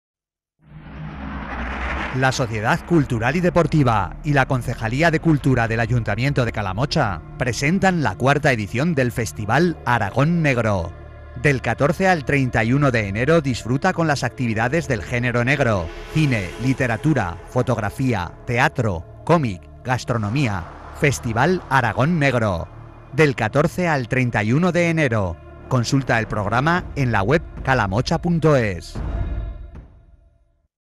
Spot: Festival Aragón Negro en Calamocha 2017
La Sociedad Cultural y Deportiva y la Concejalía de Cultura del Ayuntamiento de Calamocha presentan la 4ª edición del Festival Aragón Negro. Del 14 al 31 de enero disfruta con las actividades del genero negro. Cine, literatura, fotografía, teatro, comic, gastronomía. Festival Aragón Negro, del 14 al 31 de enero, consulta el programa en la web